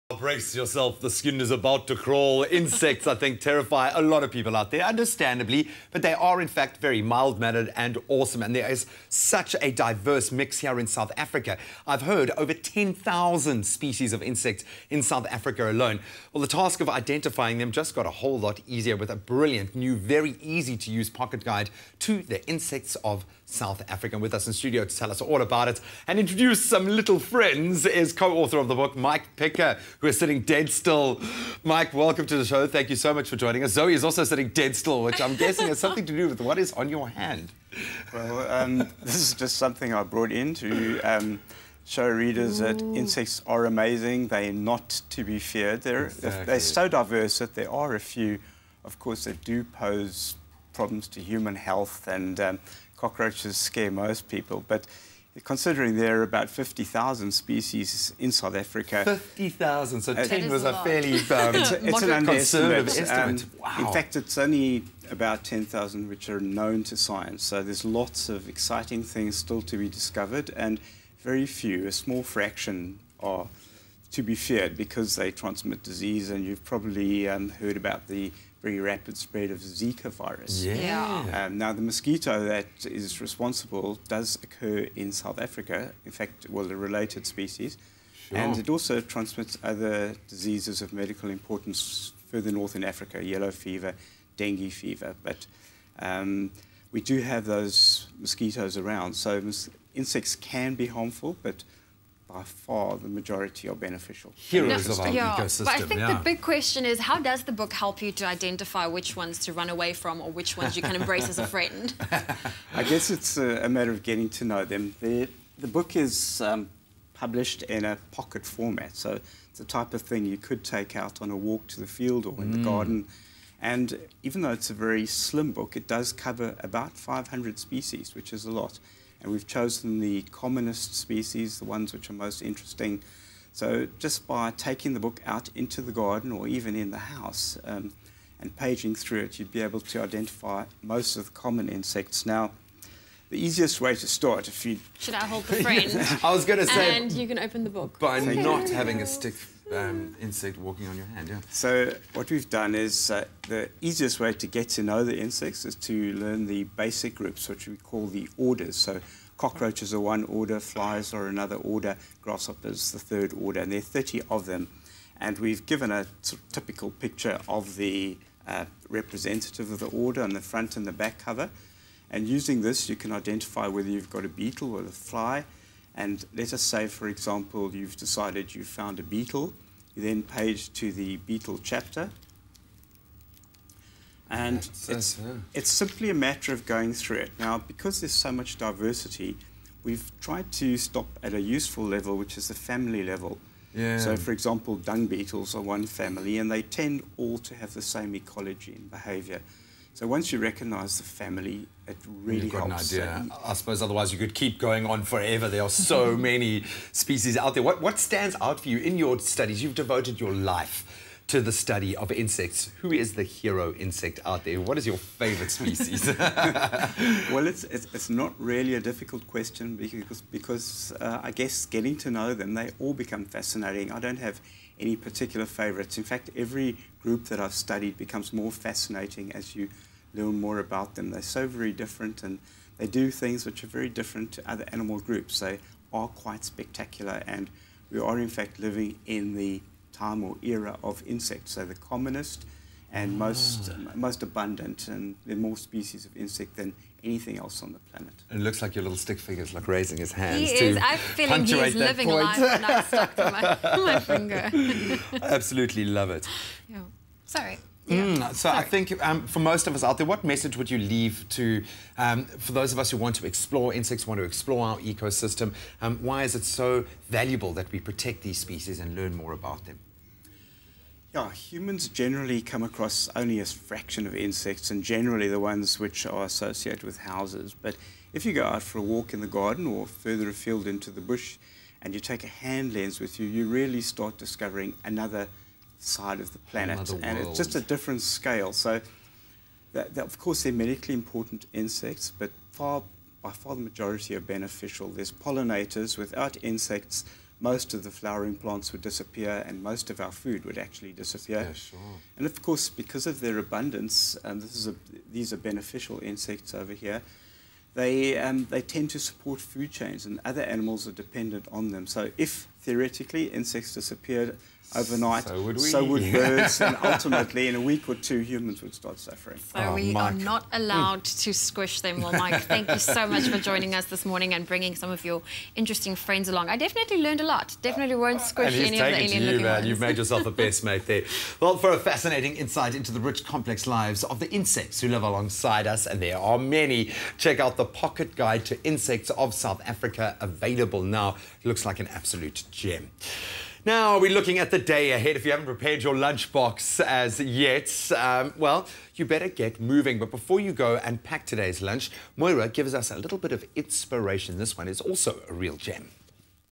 The ultimate insect identifier
Insects! For many of us, they make our skin crawl, but they are in fact some of the most fascinating organisms on our planet, and with over 10,000 insects in South Africa alone, the task of identifying them just got a whole lot easier with a new easy to use pocket guide to the insects of South Africa.